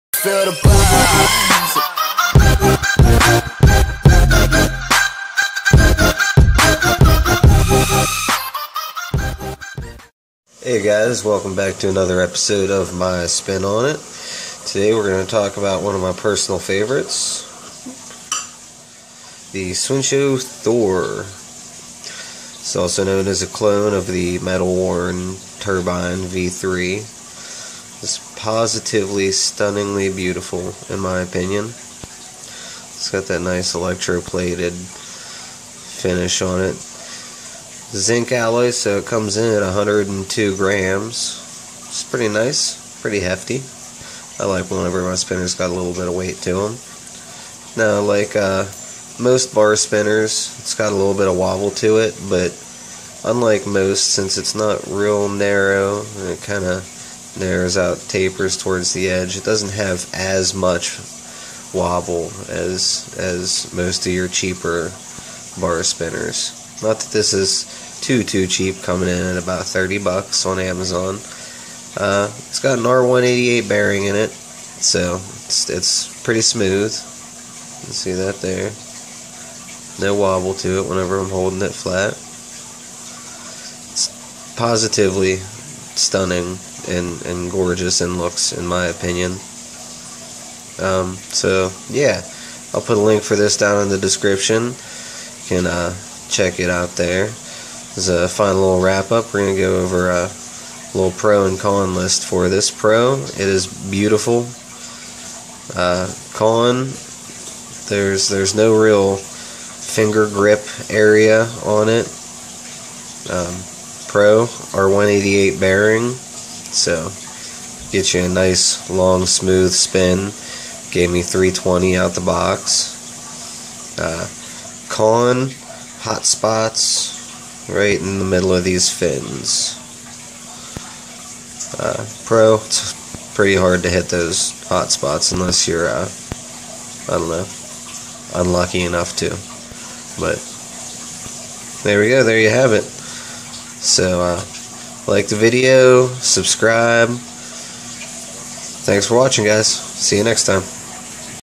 Swincho Thor (metal worn turbine clone) fidget hand spinner review by my spin on it
Thor is now in Amazon prime here

And don't forget to join the daily fidget to get in on all the other cool fidget Talk